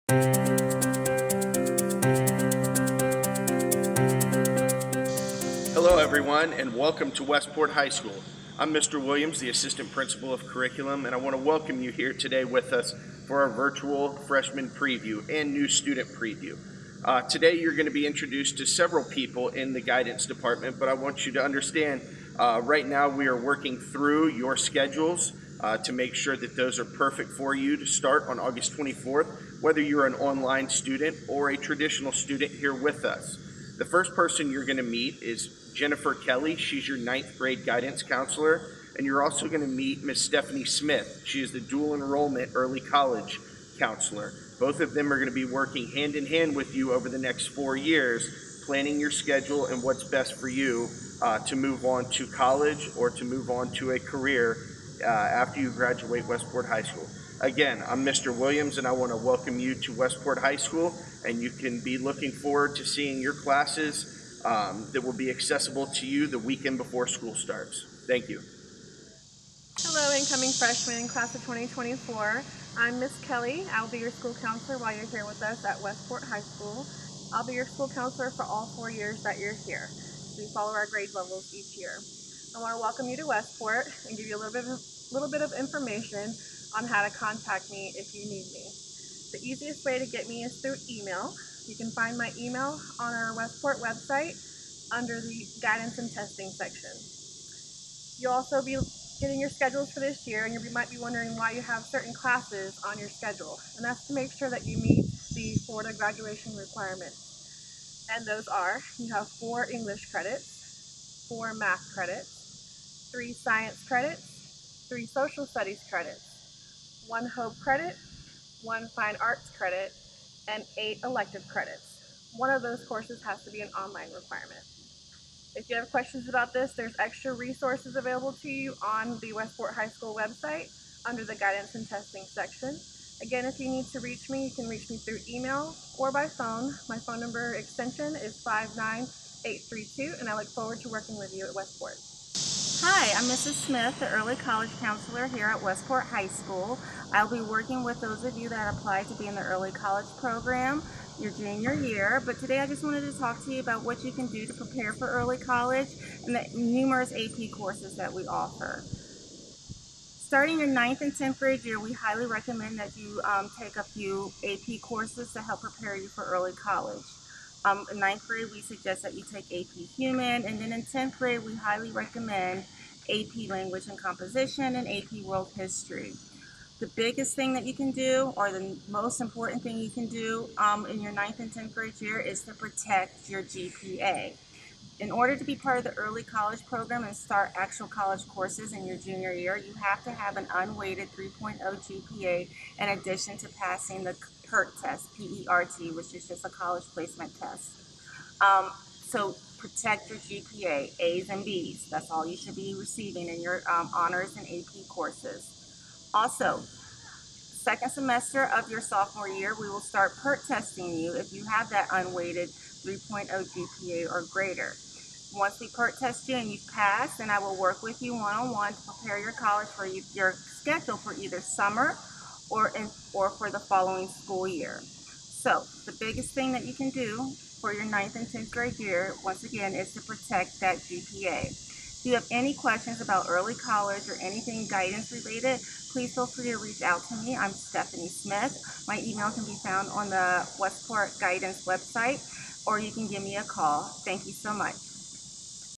Mr. Williams and Guidance Counselor's Information
Information about class schedules, graduation requirements, and early college at West Port High School in Ocala, Florida.